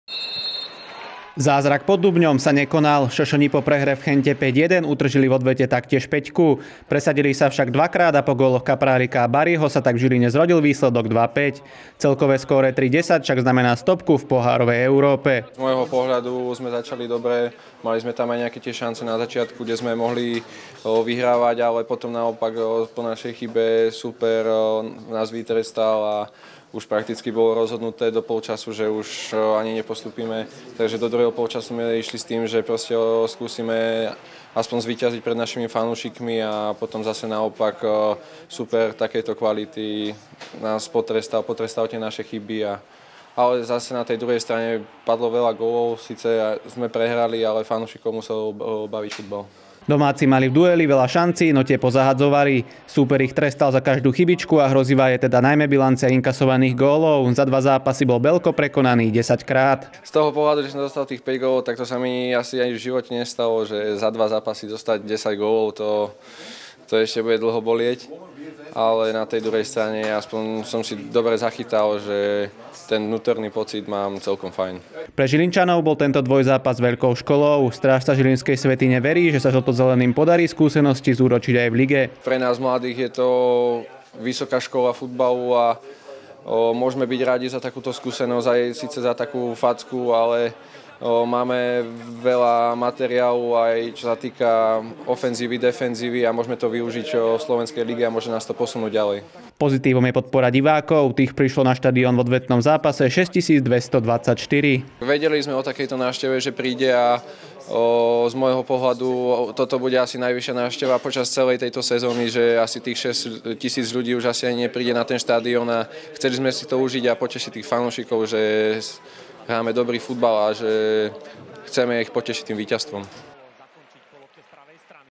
Šošoni sa lúčia s pohárovou Európou
Zázrak pod Dubňom sa nekonal. Šošoni po prehre v Gente 5:1 utržili v odvete taktiež päťku. Presadili sa však dvakrát a po góloch Kaprálika a Bariho sa tak v Žiline zrodil výsledok 2:5. Celkové skóre 3:10 však znamená stopku v pohárovej Európe.